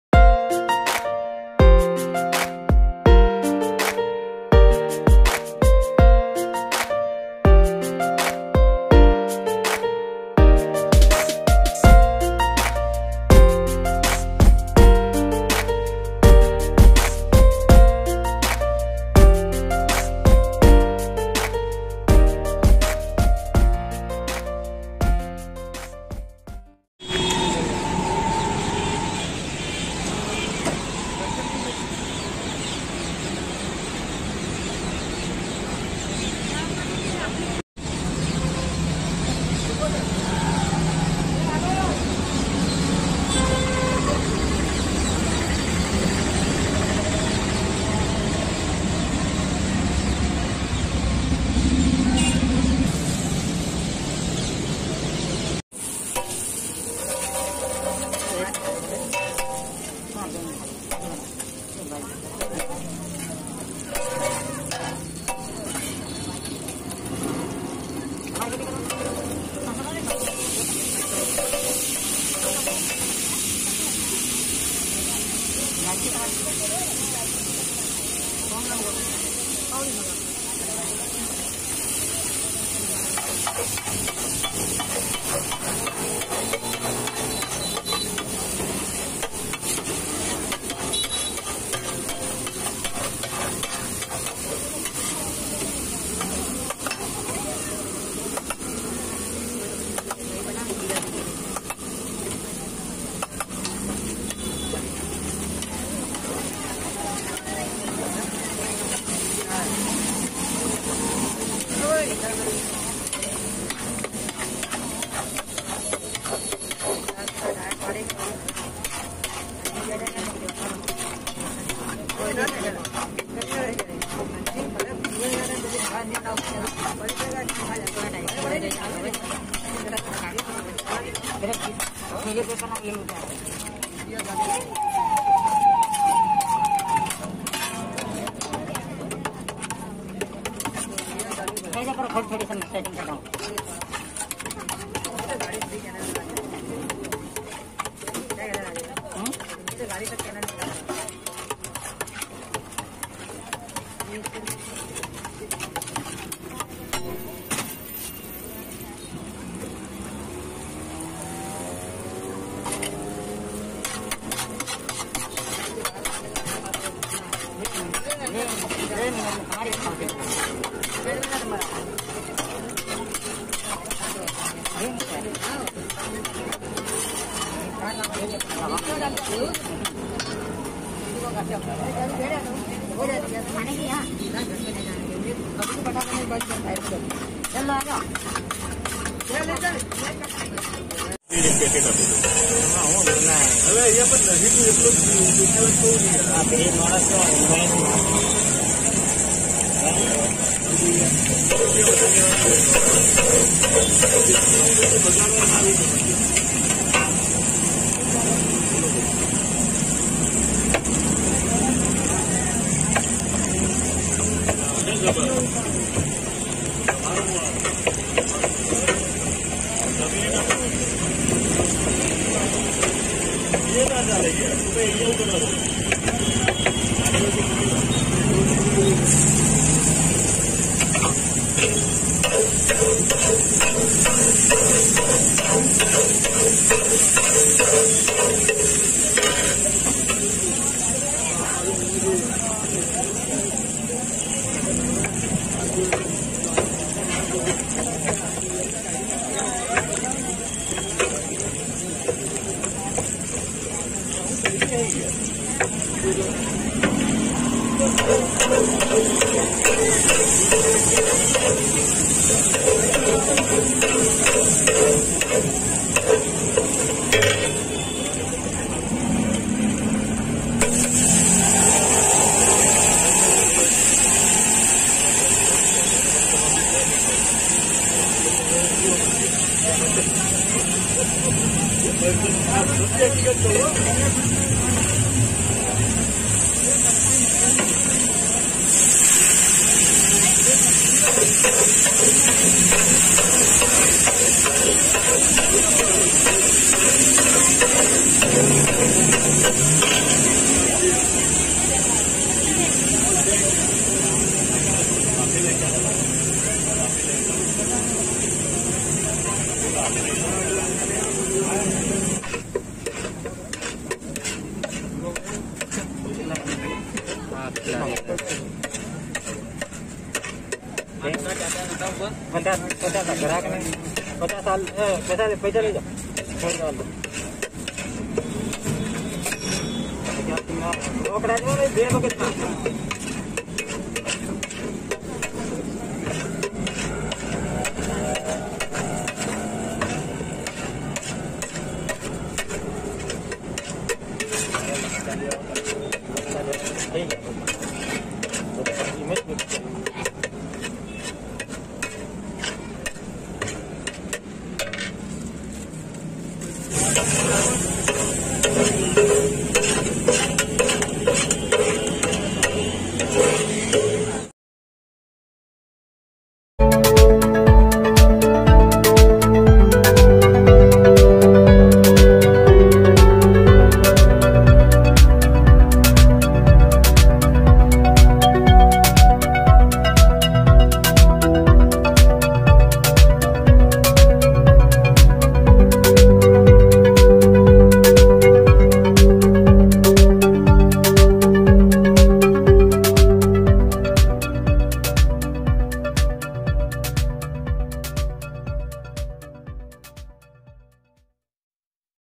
Krushna Pavbhaji | Indian Street Food | Mehsana
Pav bhaji is a fast food dish from Maharashtra consisting of a thick vegetable curry served with a soft bread roll.

Raju bhai is standing here with food trolley from last 10 years. He has amazing taste in pav bhaji as well as pulav.

Krushna pavbhaji
Address:- Opp- T. J. High school, Mamlatdar kacheri road, Mehsana.
Timing:- 5 PM to 10 PM